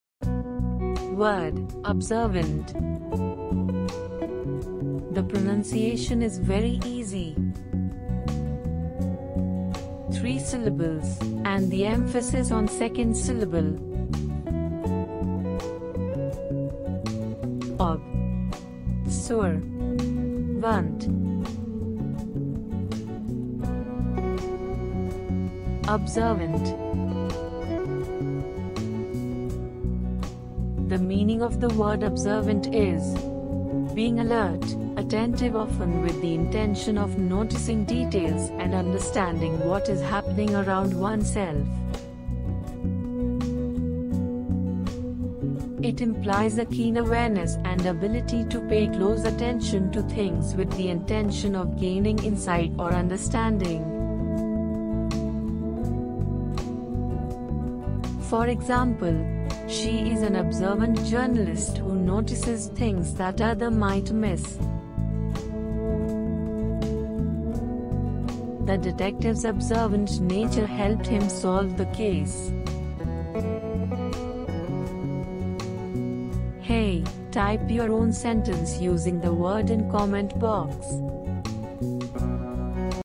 Tool to Remember Forever : English Word "OBSERVANT" | Vocabulary
In this video, we'll be learning the English word "OBSERVANT"   We'll be using the Visual Example Tool to help us remember this word.

In the next video tomorrow, we'll be learning the English word "TRUSTWORTHY"

Expand you Word Vocabulary with Examples

Explore other learning videos through series of playlists mentioned below.